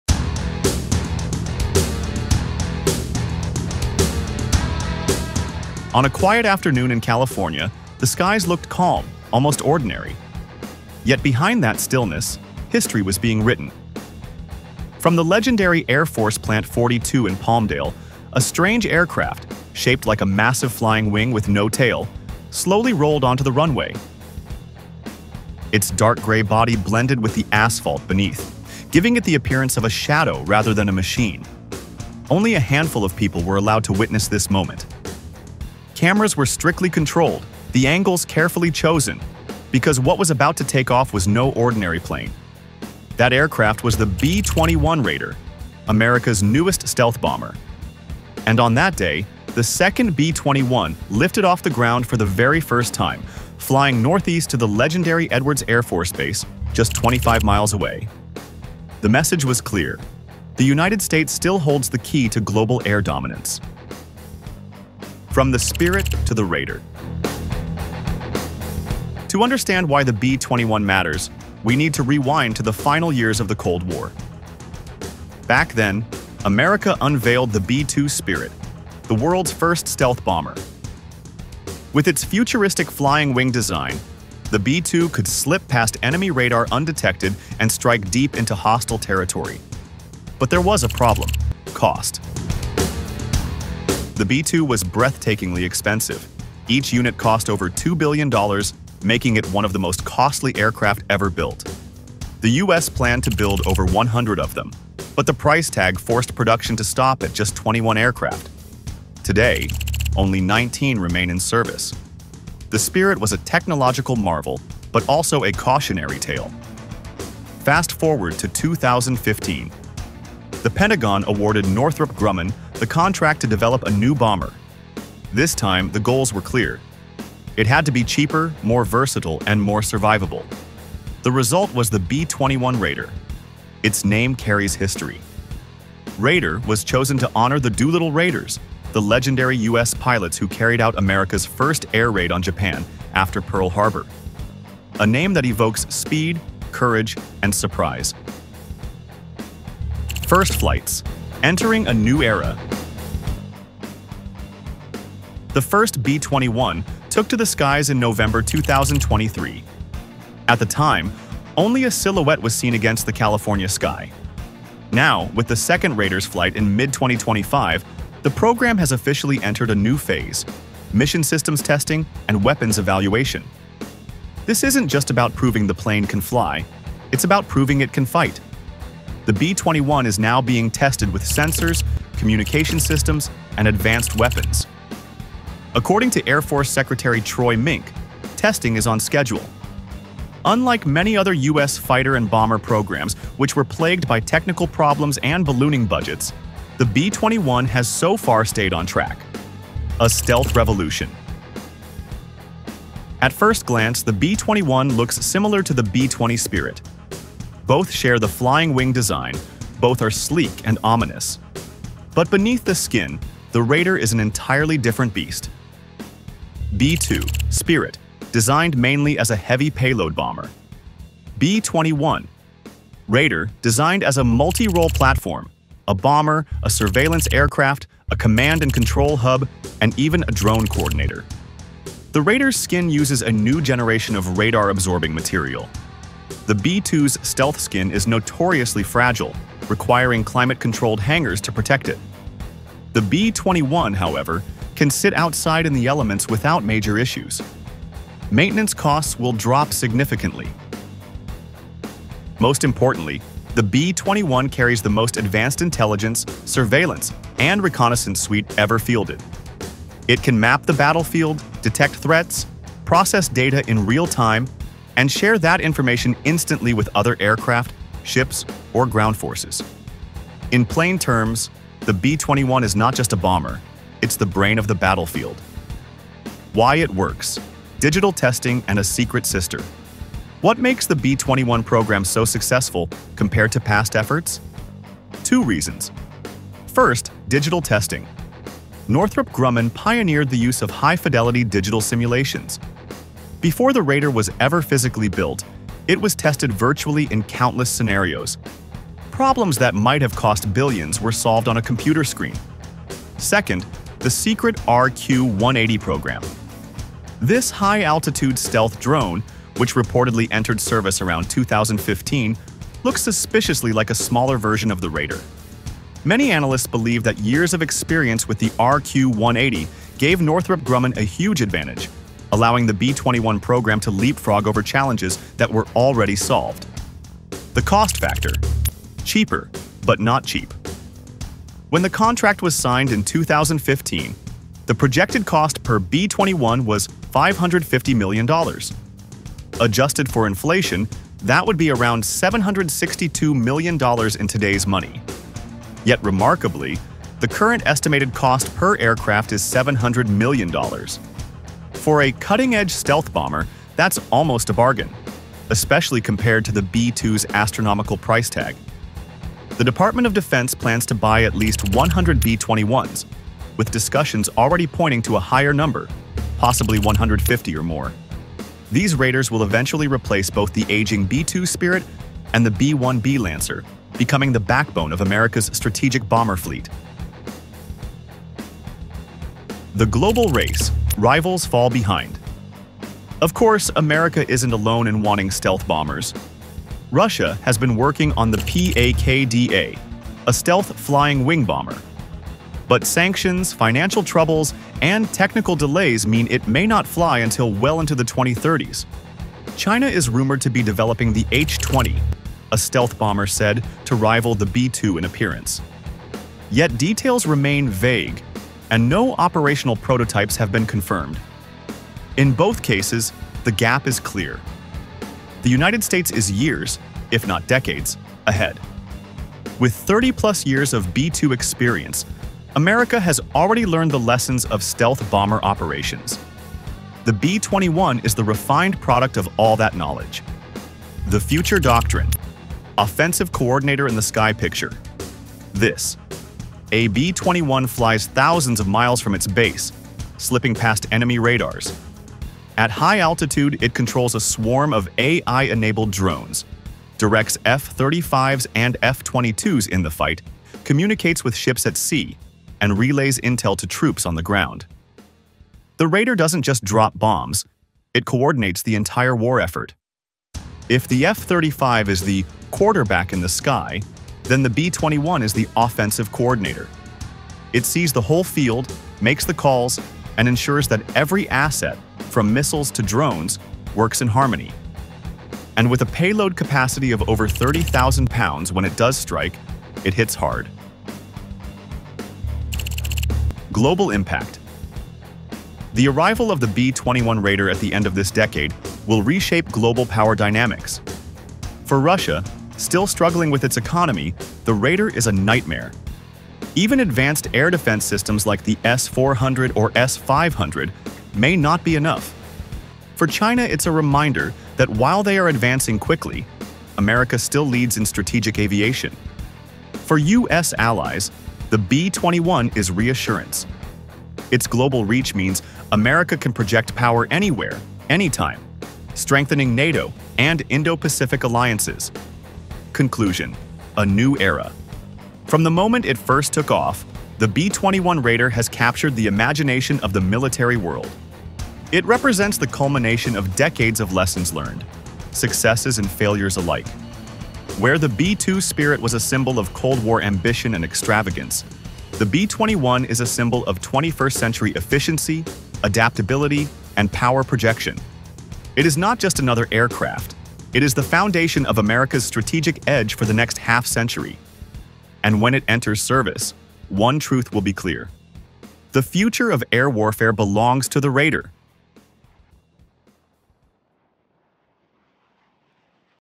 Why the B-21 Raider Could Change Air Power Forever
America's 2ND B-21 Bomber is now flying

The U.S. Air Force has officially begun flight tests with its second B-21 Raider stealth bomber. This marks another major step in America’s push to modernize its long-range strike capabilities.

Developed by Northrop Grumman, the B-21 is designed to be the most advanced stealth bomber ever built — capable of carrying both conventional and nuclear weapons, evading even the most sophisticated enemy air defenses, and striking targets anywhere in the world.

With the second Raider now taking to the skies, the program is moving closer to becoming operational, and military analysts are watching closely to see how this next-generation bomber will change the balance of power.